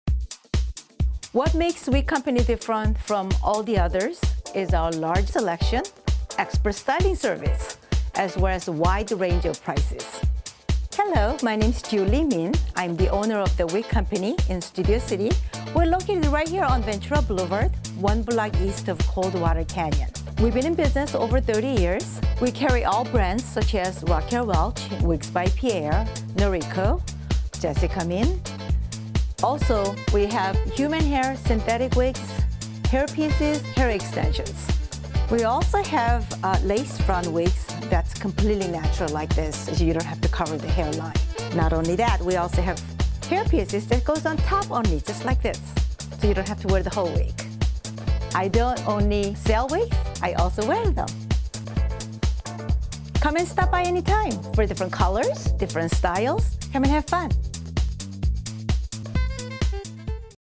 The Wig Company - Wig Shop in Studio City
The Wig Company has been in business for over 30 years. We specialize in following. 1) We work very closely with entertainment industry providing wigs and hair pieces for major motion pictures and TV shows such as 'Desperate Housewives", "Angels" "Brothers and Sisters". Many A-List clients in the entertainment industry. 2) We have largest selection for Chemotherapy patients. We work very closely with most of the local hospitals and doctors for many years, providing the most comfortable and natural looking wigs. 3) We provide expert styling service. Because wigs and hairpieces are such a personal item, we custom cut and shape each item to fit the individual customer. We also provide "wash and set" styling service for your own wigs. 4) Extremely friendly and knowledgeable stylists waiting to serve you.